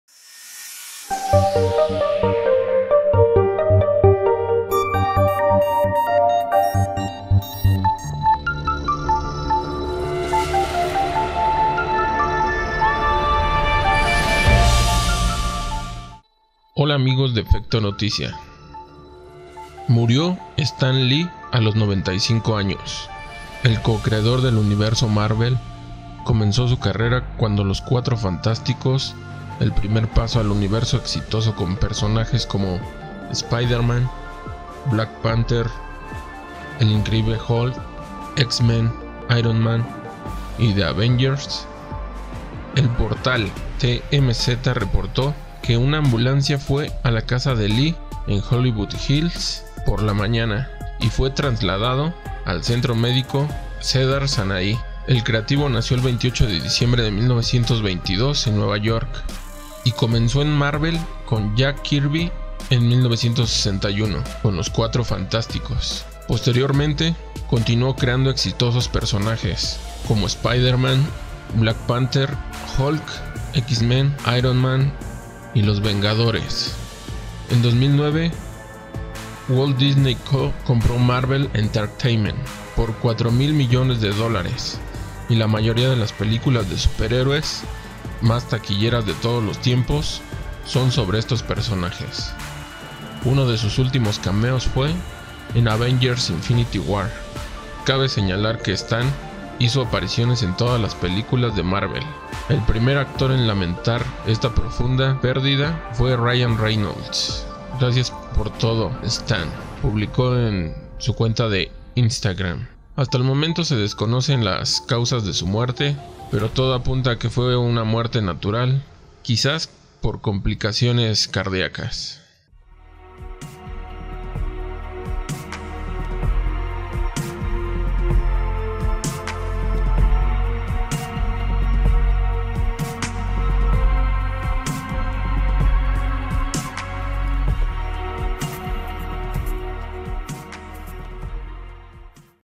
STAN LEE HA MURT0 12/11/2018
StanLee muri0 a los 95 años de edad.